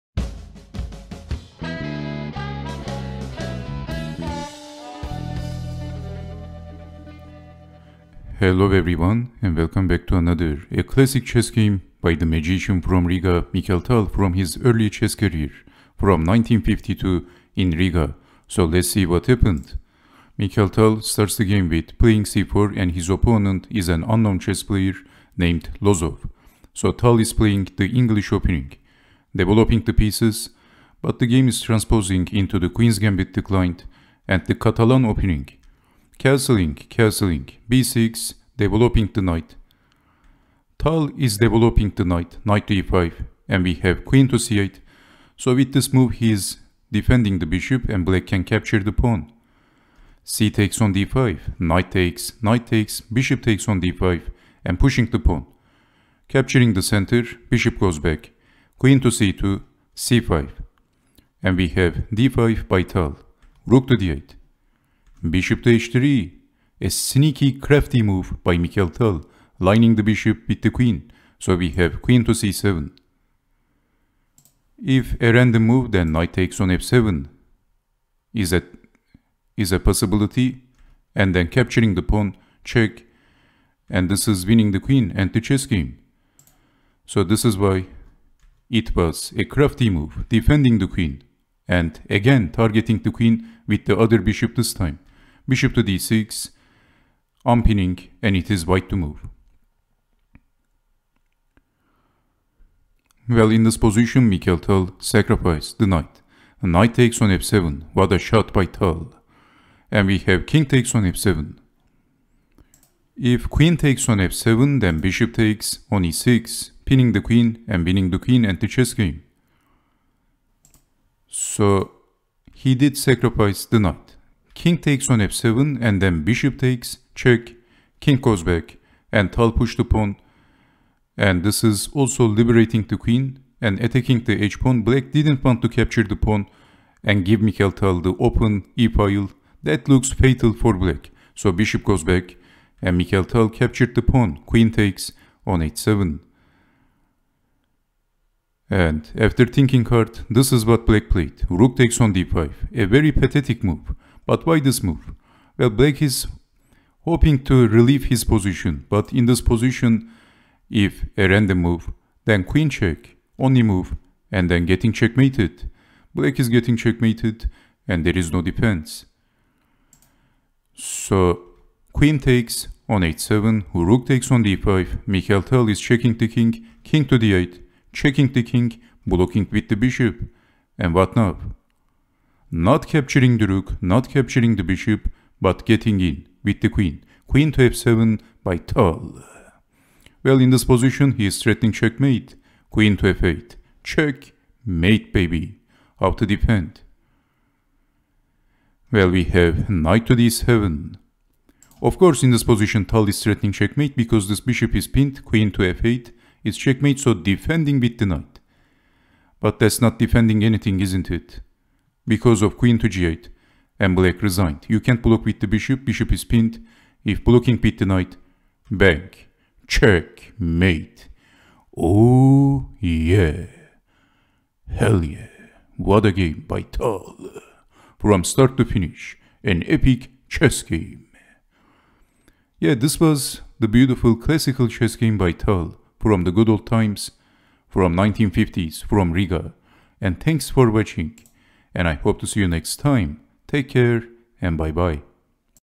An Epic Chess Game by Tal From Riga 1952
Mikhail Tal (White)

Lozov (Black)

Riga (1952)

Queen's Indian Defense Classical. Traditional Variation Nimzowitsch Line